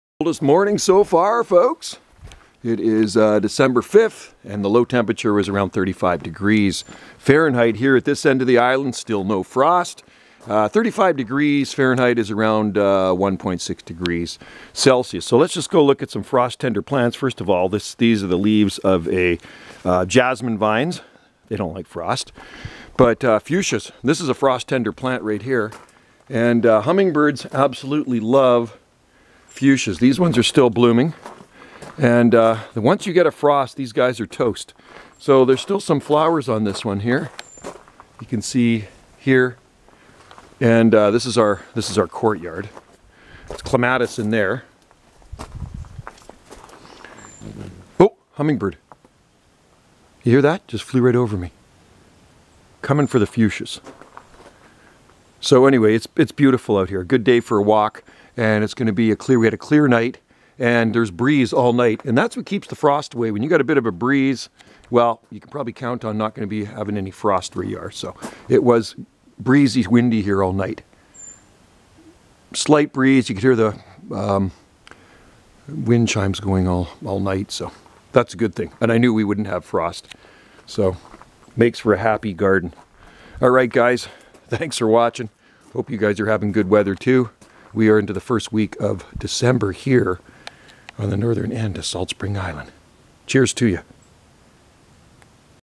Clear cool December morning doesn't stop Hummingbirds on Salt Spring Island
Beautiful clear & cool frost free morning in our exotic garden